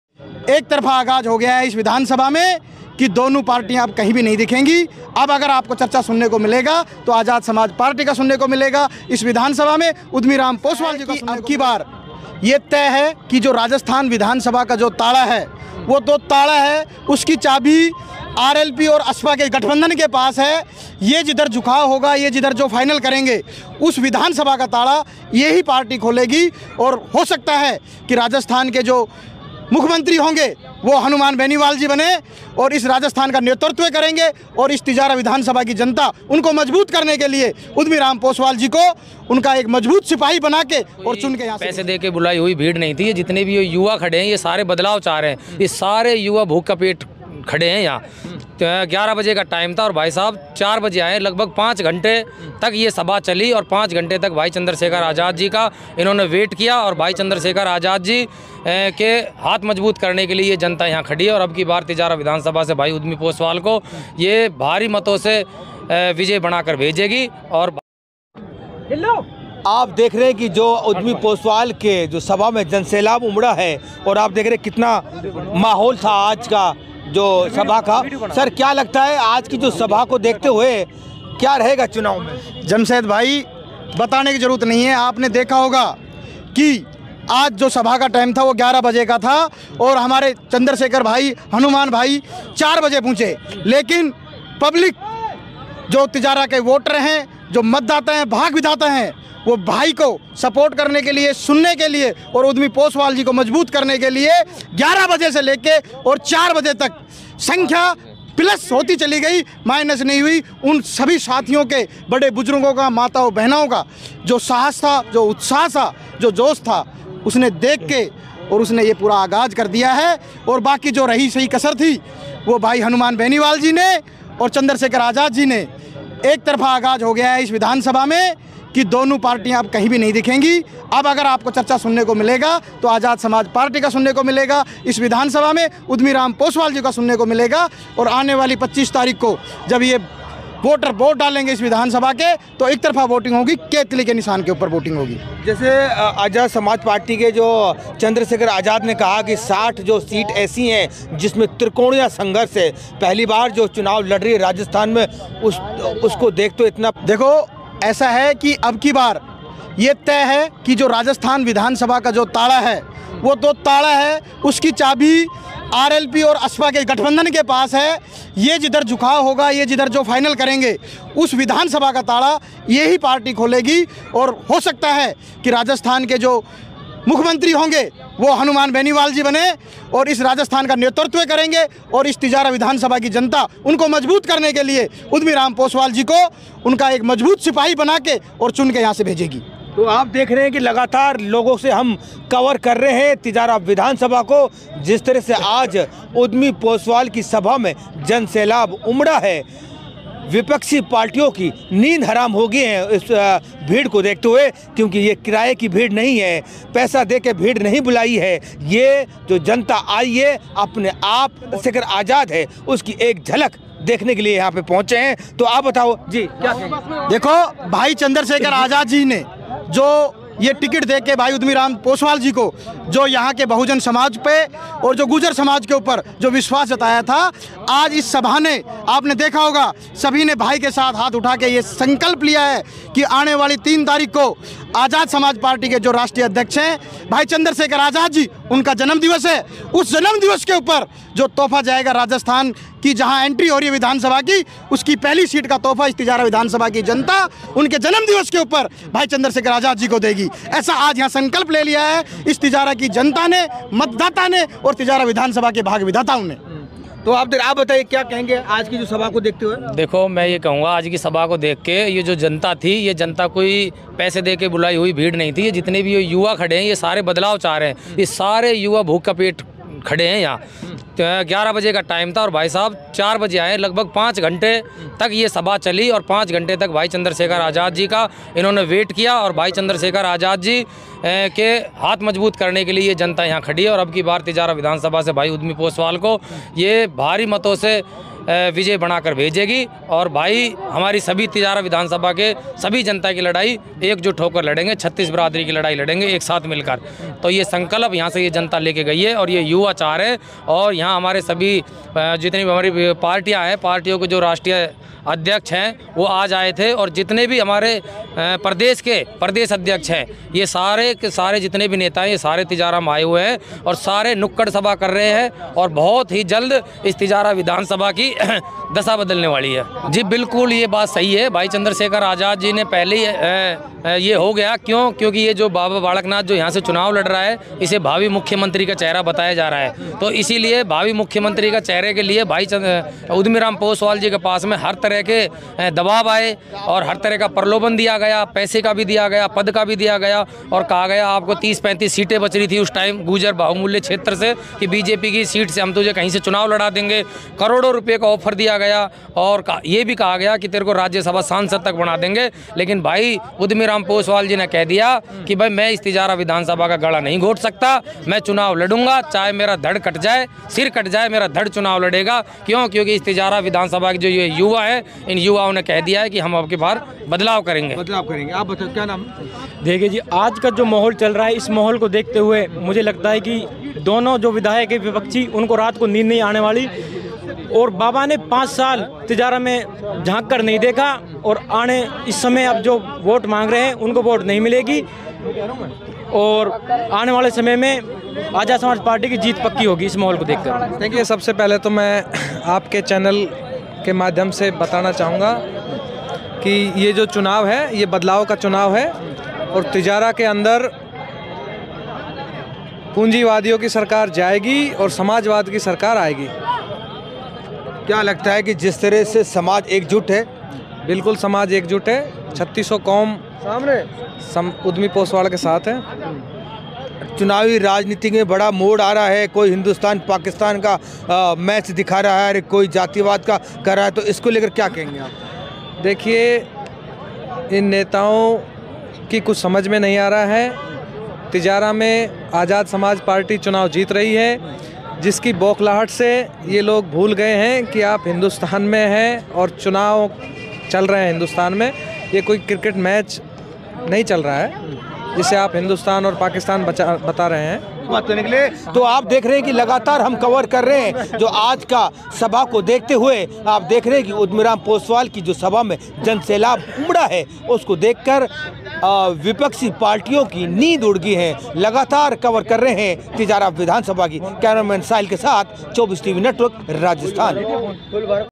बाबा बालक नाथ को जवाब देने के लिए पहुंचे तिजारा चंद्रशेखर आजाद
संदीप दायमा
Mahant Balak Nath
Maman Singh Yadav
BMA bhiwadi
Jamshed khan